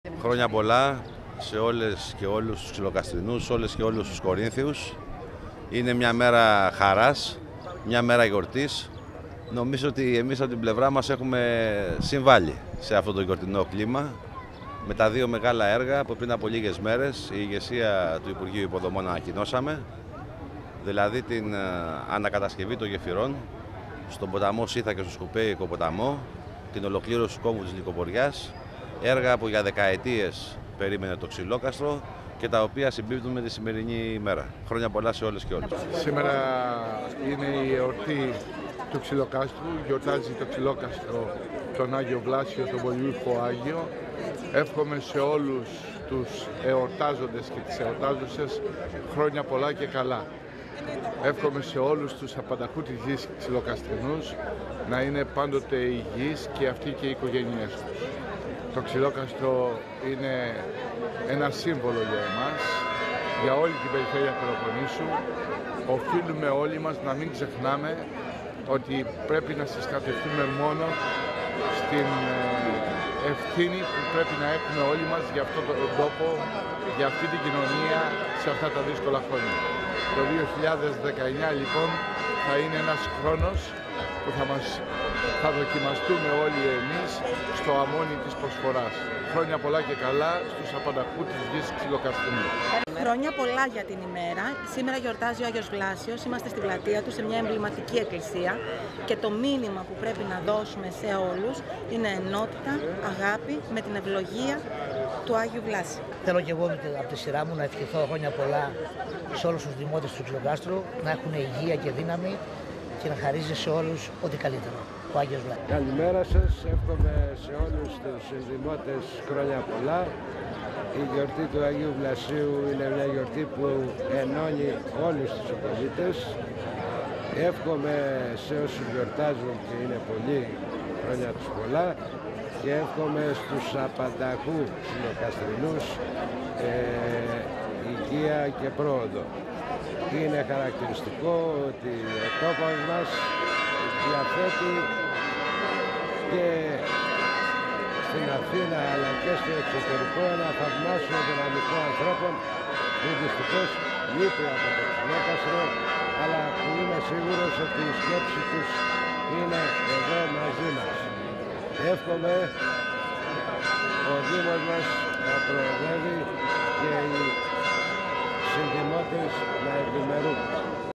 Εορτασμός Αγίου Βλασίου Ξυλοκάστρου
ΔΕΙΤΕ ΠΕΡΙΣΣΟΤΕΡΑ ΕΔΩ:
ΜΕ ΛΑΜΠΡΟΤΗΤΑ ΕΟΡΤΑΣΤΗΚΕ Η ΜΝΗΜΗ
ΤΟΥ ΠΟΛΙΟΥΧΟΥ ΞΥΛΟΚΑΣΤΡΟΥ ΑΓ. ΙΕΡΟΜΑΡΤΥΡΟΣ ΒΛΑΣΙΟΥ
ΜΕ ΤΗΝ ΠΑΡΟΥΣΙΑ ΤΟΥ ΣΕΒ. ΜΗΤΡΟΠΟΛΙΤΟΥ ΠΑΤΡΩΝ κ. ΧΡΥΣΟΣΤΟΜΟ